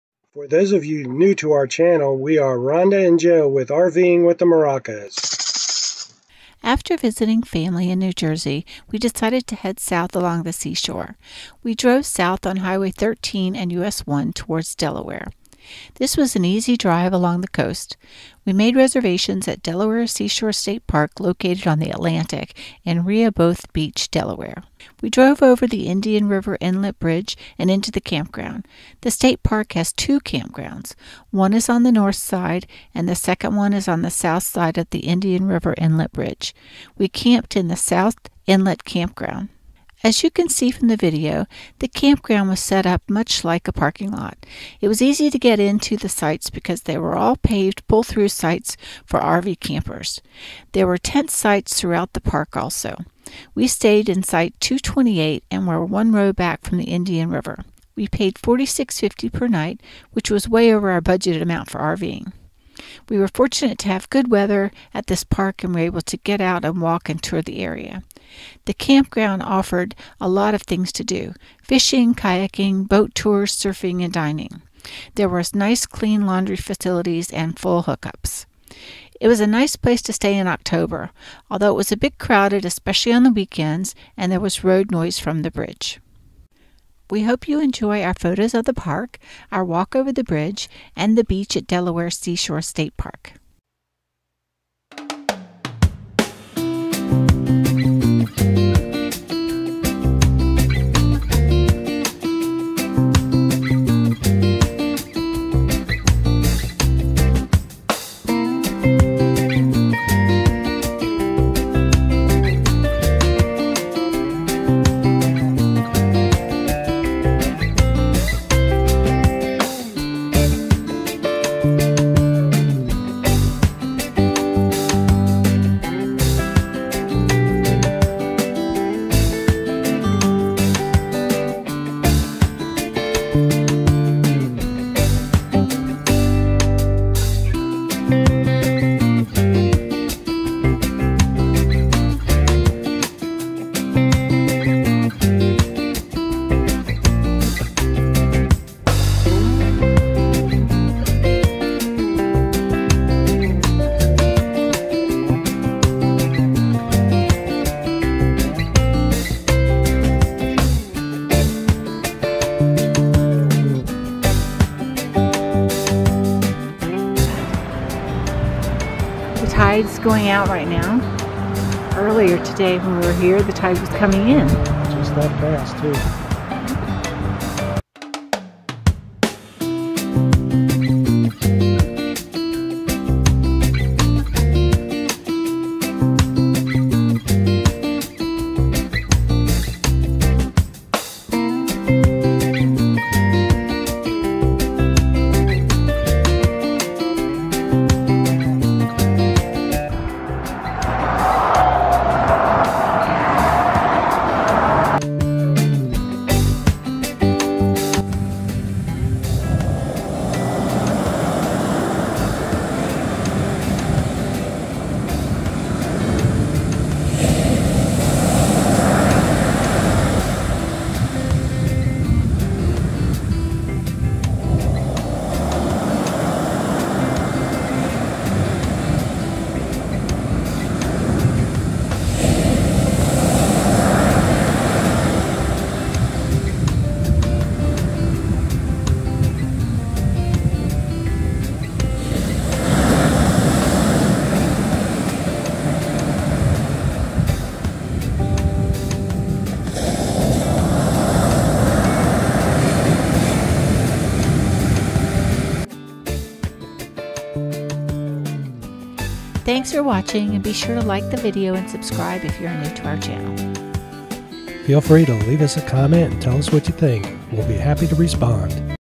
Delaware Seashore State Park
October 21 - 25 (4 nights)

After visiting family in NJ, we decided to head south along the seashore.  We drove south on Highway 13 and US 1 towards Delaware.  This was an easy drive along the coast.  We made reservations at Delaware Seashore State Park located on the Atlantic in Rehoboth Beach, DE.

We drove over the Indian River Inlet bridge and into the campground.  The state park has two campgrounds.  One is on the north side and the second is on the  south side of the Indian River Inlet Bridge.  We camped in the south inlet campground.

As you can see from the video, the campground was set up much like a parking lot.  It was easy to get into the sites because they were all paved pull-through sites for RV campers.  There were tent sites throughout the park also.  We stayed in site 228 and were one row back from the Indian River.  We paid $46.50 per night, which was way over our budgeted amount for Rving.  We were fortunate to have good weather at this park and were able to get out and walk and tour the area.  The campground had offered alot of things to do;  fishing, kayaking, boat tours, surfing, and dining.  There were nice clean laundry facilities.